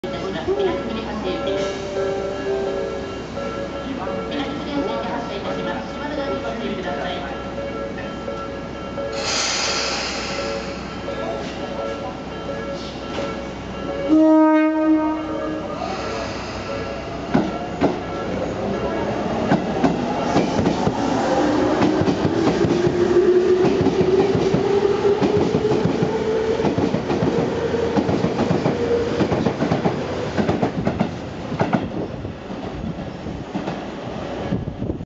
[警笛あり]東武鉄道20050系21857F日比谷線直通東武ｽｶｲﾂﾘｰﾗｲﾝ 竹ノ塚駅発車
東武鉄道20050系21857F日比谷線直通東武ｽｶｲﾂﾘｰﾗｲﾝの竹ノ塚駅発車シーンです。
保線作業員に向けての警笛です。2016.6.8撮影